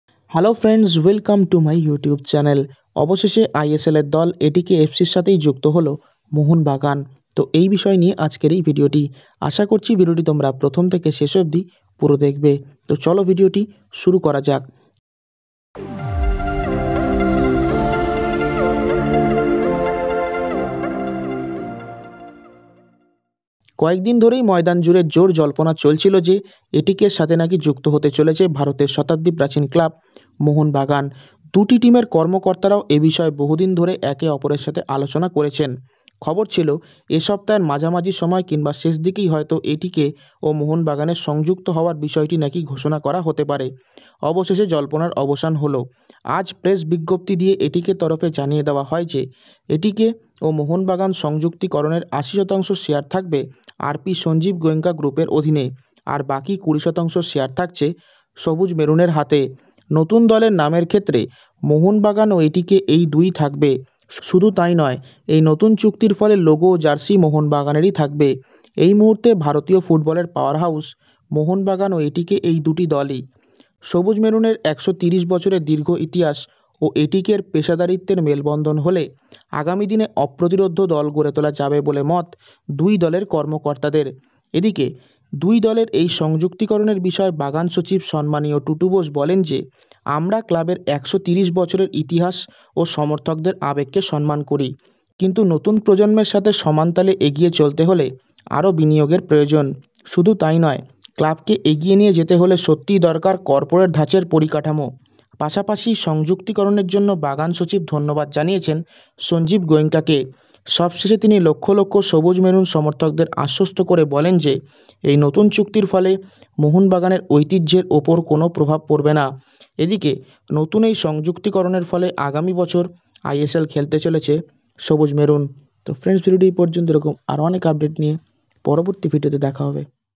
সব জল্পনার অবসান ঘটিয়ে আজ এটিকের সাথে যুক্ত হল ভারতের ঐতিহ্যশালী ক্লাব মোহনবাগান।
● ভিডিওটির বিষয় : আগামী বছর আইএসএল খেলার আগে আজ এটিকের সাথে যুক্ত হল মোহনবাগান।